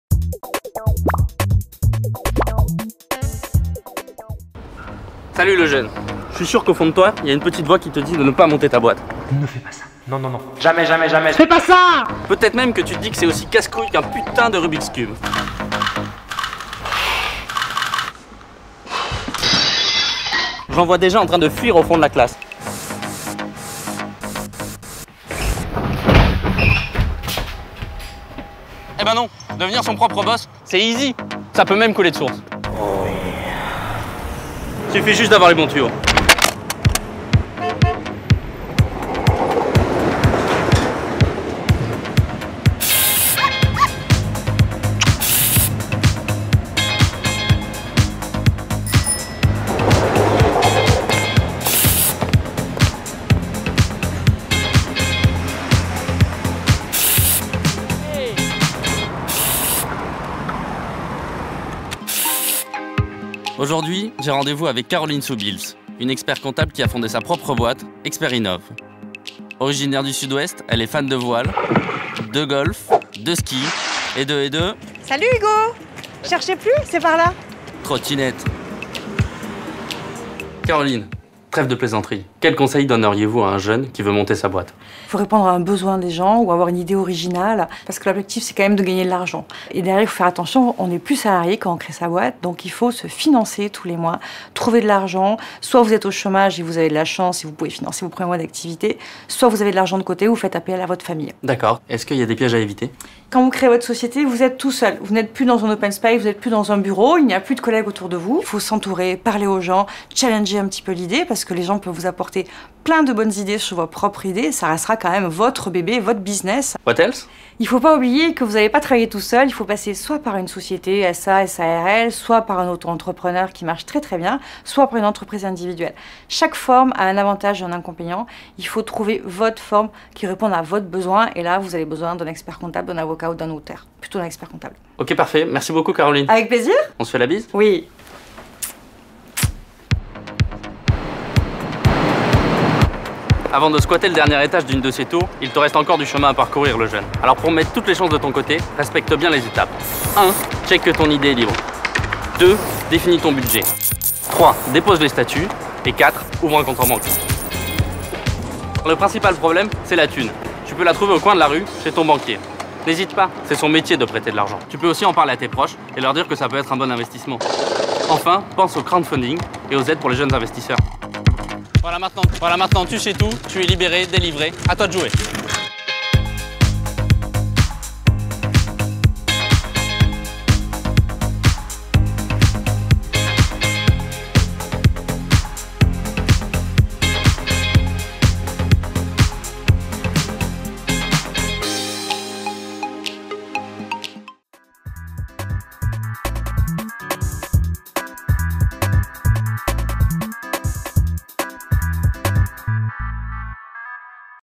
Hugo les bons tuyaux - comment créer ma boite ?
Monter sa boite ? Voilà qui a de quoi effrayer le plus motivé des entrepreneurs en herbe. Pourtant avec les conseils d'Hugo les bons tuyaux, tout devient bien plus simple.